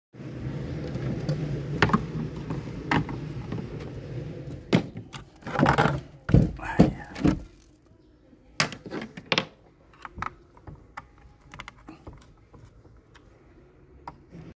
Remote excavator is defective, repair the excavator and clean and clean the dirt inside the machine3
Remote excavator is defective, repair the excavator and clean and clean the dirt inside the machine